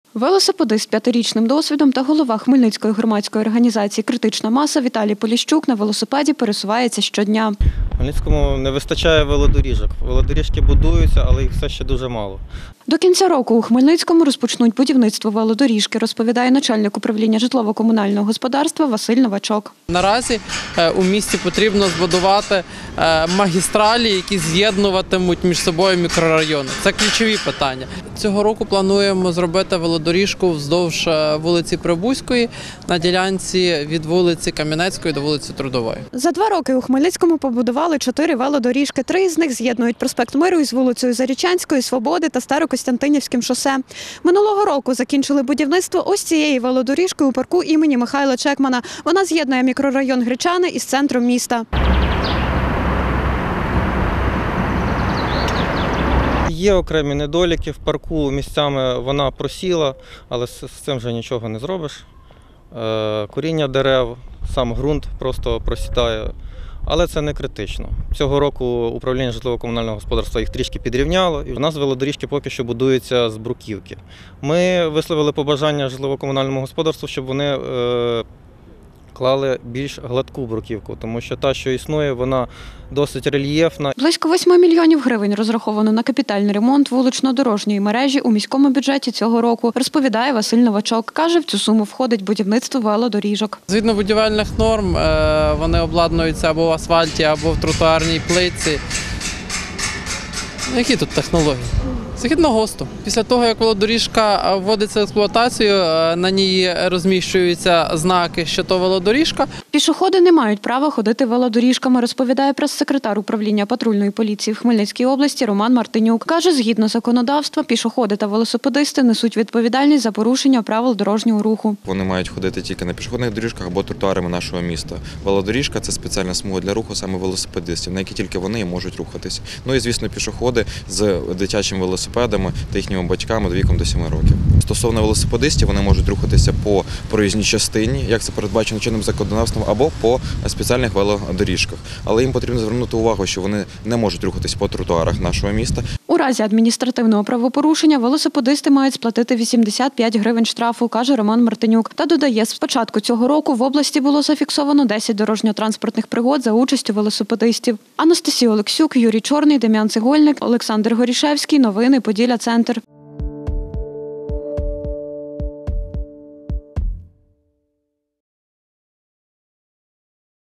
Нова велодоріжка з’явиться у місті Хмельницькому
Нова велодоріжка з’явиться у місті Хмельницькому. Її будівництво планують розпочати цього року. За словами начальника міського управління житлово-комунального господарства Василя Новачка, велодоріжка з’єднуватиме вулицю Кам’янецьку та Трудову.